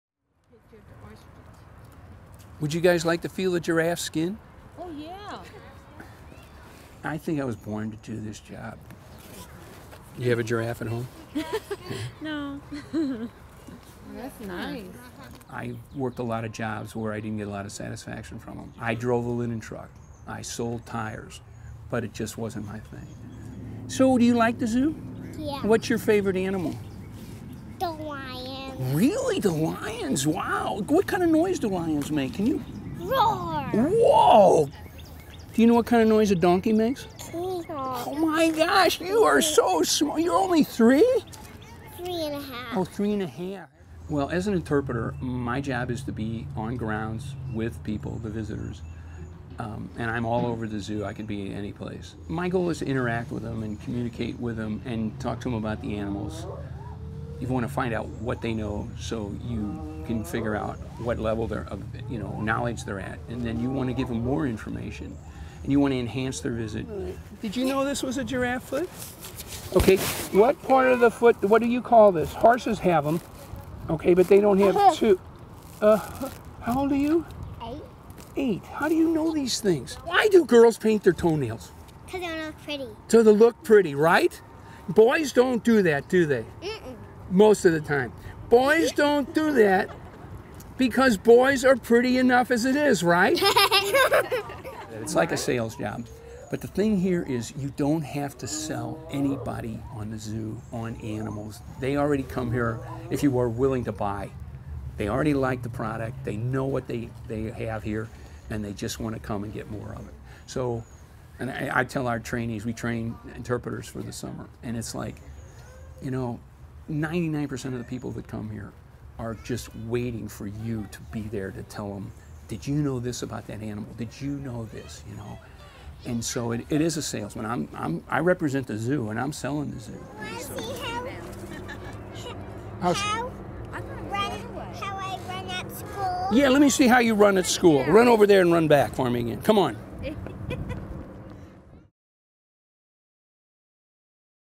St. Louis Zoo interpreter brings animals closer
The St. Louis Zoo's only full time interpreter, Jerry DiMarcellis, entertains and educates visitors. He has held the position for nearly a decade.